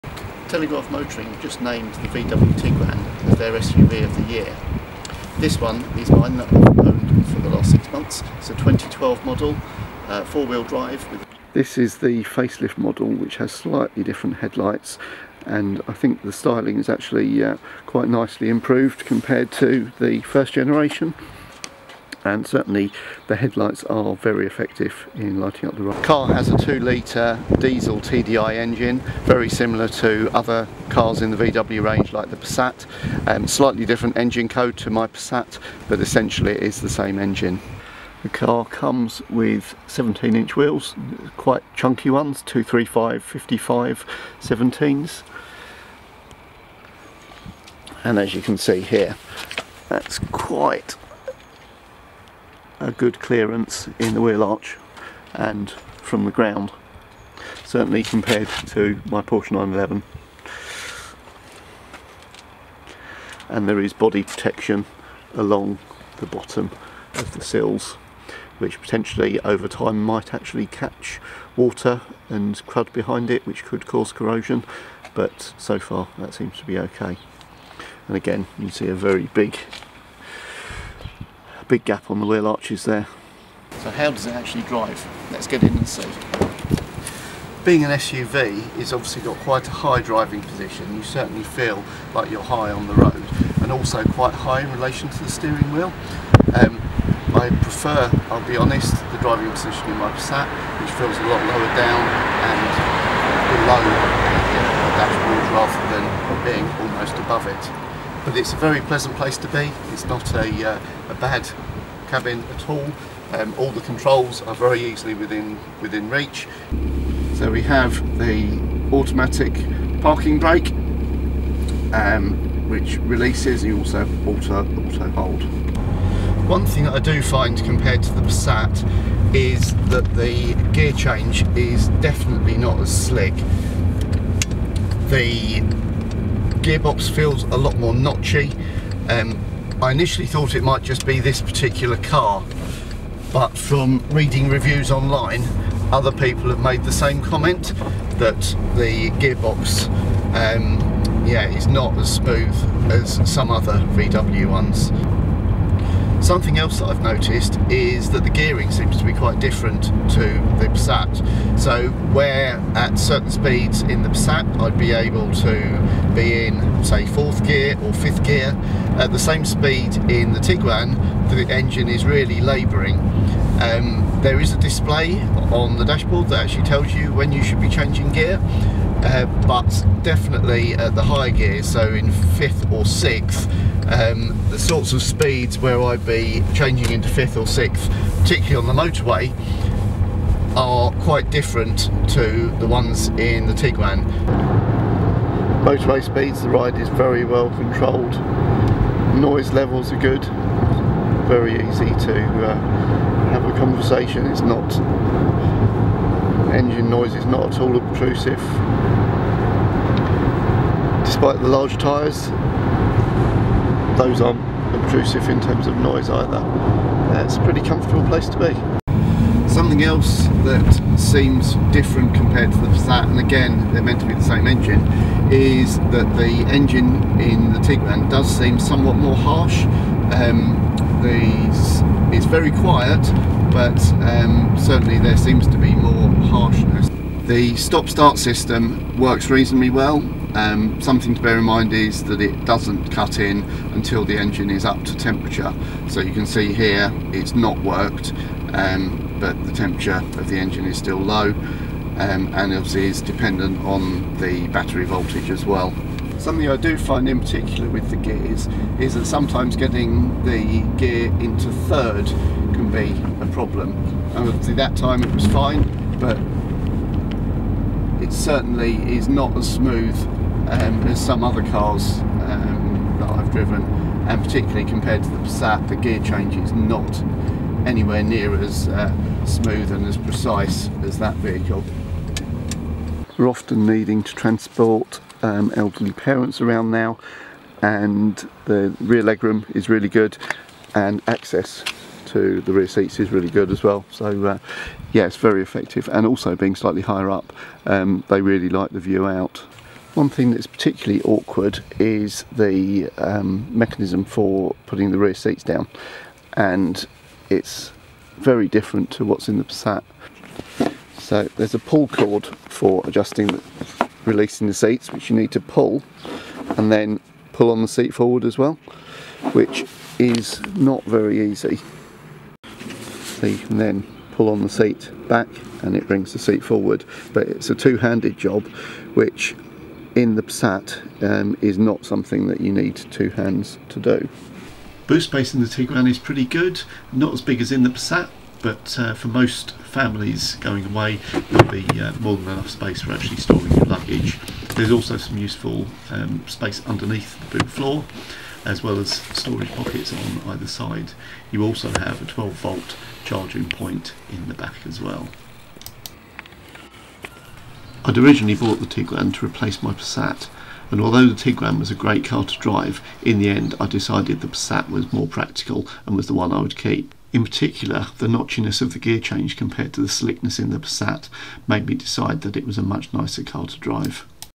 VW Tiguan 2012 TDI Review - 2024 review. What's it like 12 years on?
I bought my VW Tiguan with the intention of replacing my older VW Passat. in the end my Passat stayed and I sold the Tiguan. Watch the video to find out why!